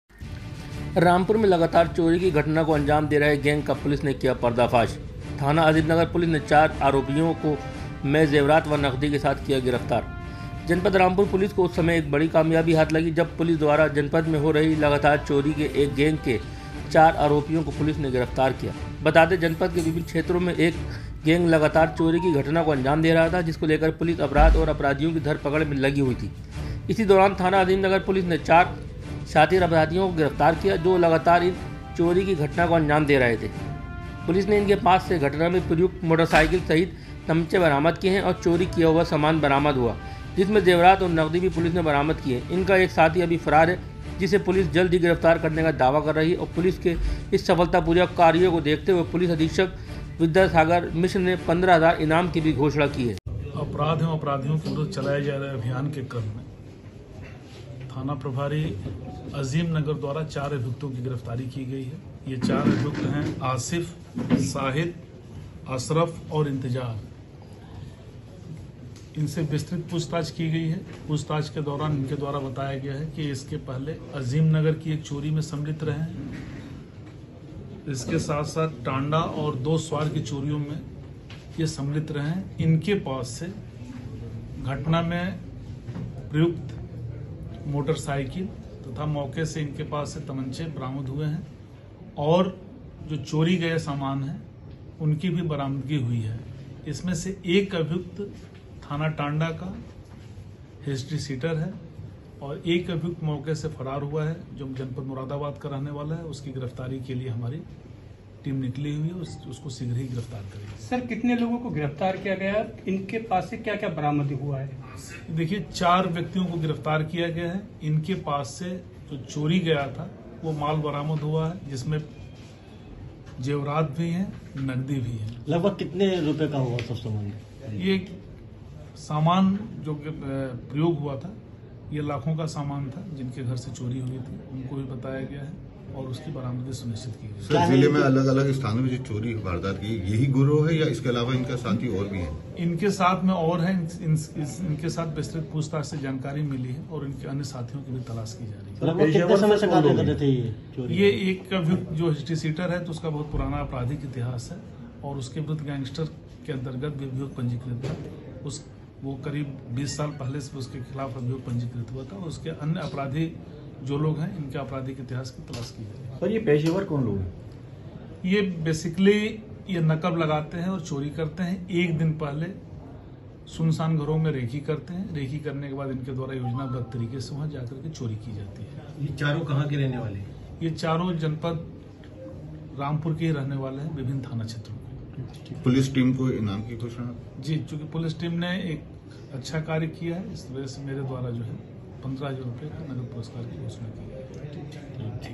Rampur:चोरी के गैंग का SP ने किया ख़ुलासा चार बदमाशो को चोरी के समान के साथ पुलिस ने किया गिरफ़्तार
चोरी के गैंग का SP ने किया ख़ुलासा चार बदमाशो को चोरी के समान के साथ पुलिस ने किया गिरफ़्तार